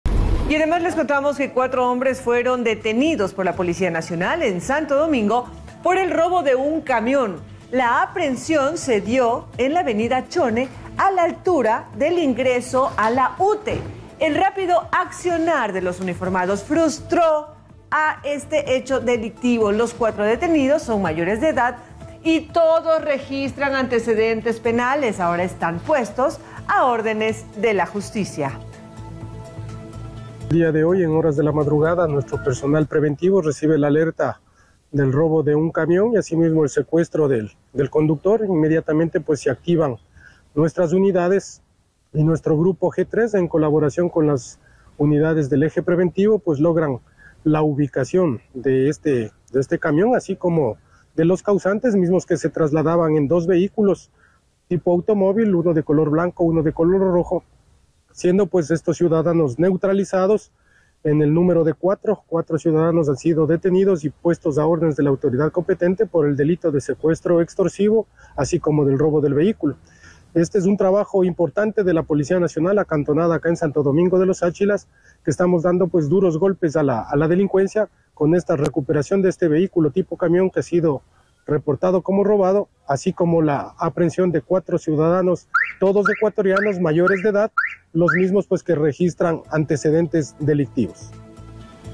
4 DETENIDOS EN DELITO FLAGRANTE
Cuatro hombres fueron detenidos por la Policía Nacional en Santo Domingo por el robo de un camión. La aprehensión se dio en la Avenida Chone, a la altura del ingreso a la UTE.